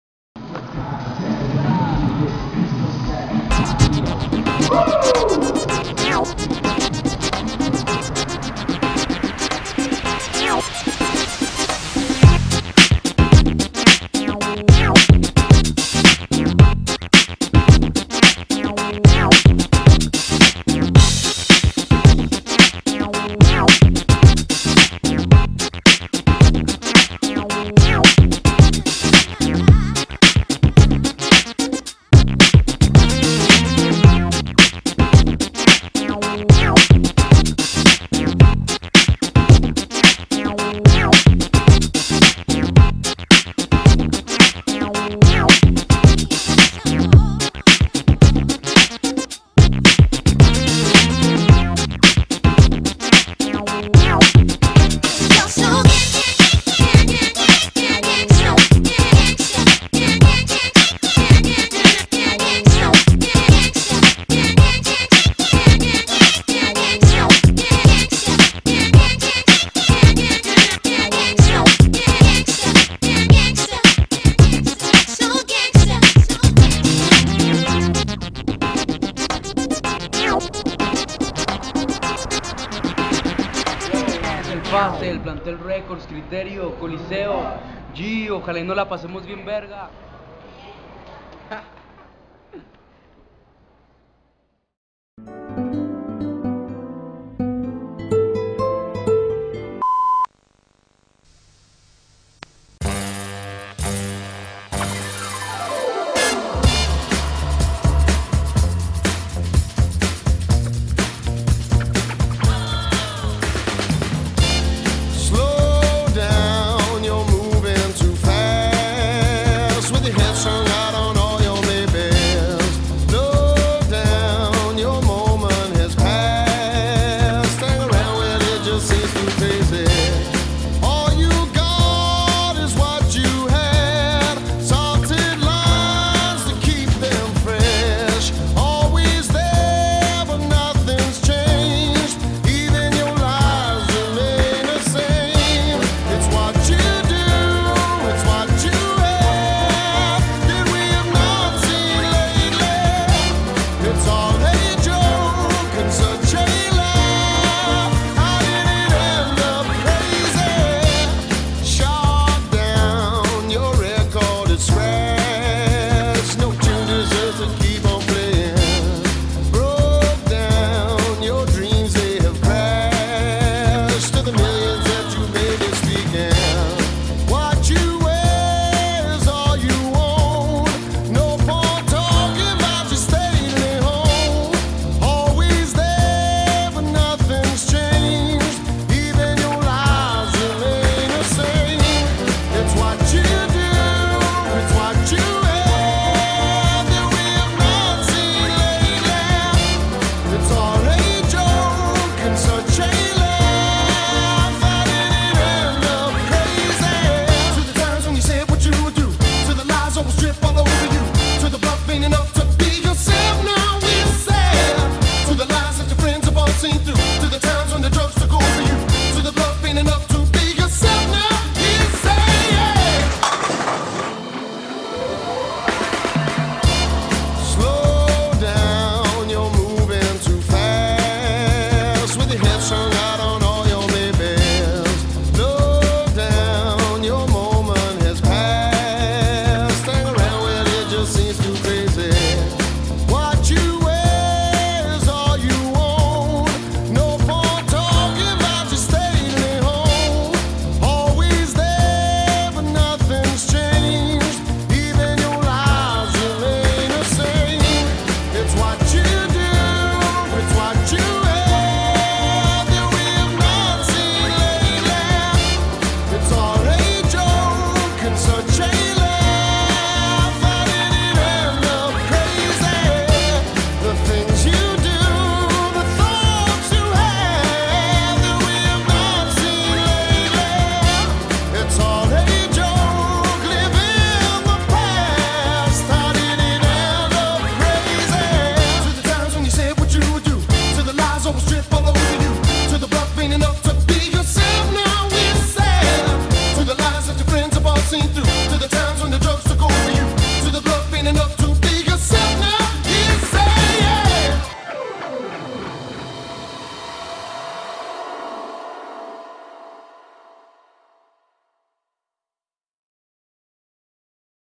Coliseo Street Chihuahua
un poco de lo que paso este mes en el coliseo street en chihuahua. espero les agrade y gracias a todos los que asistieron!